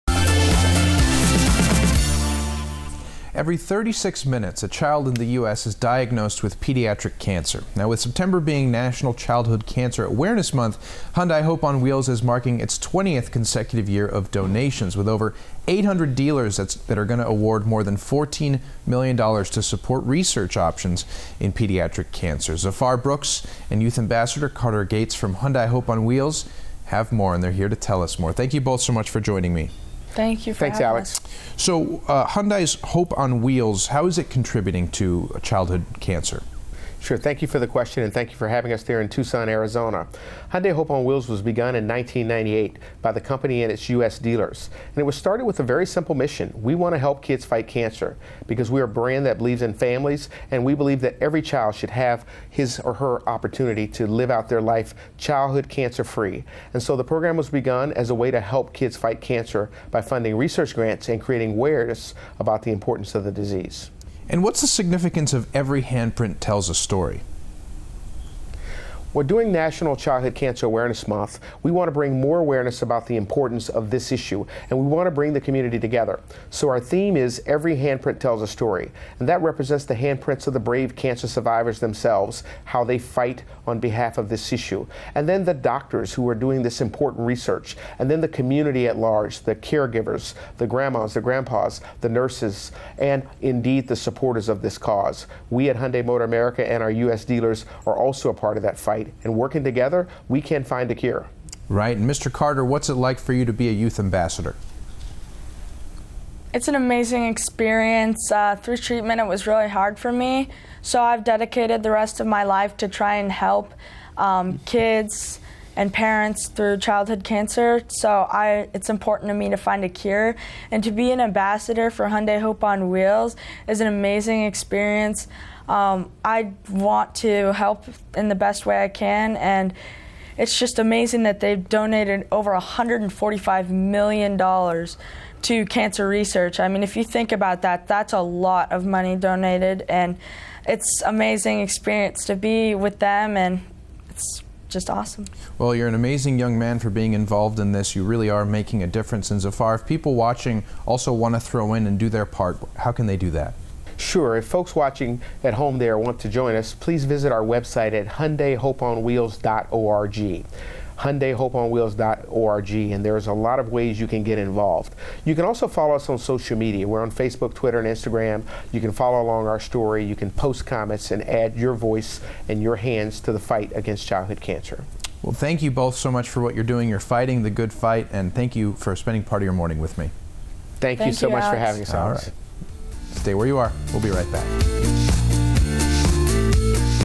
Hyundai Hope on Wheels fights childhood cancer
Hyundai’s Hope on Wheels Chairman and Youth Ambassadors available to talk about how the program makes a difference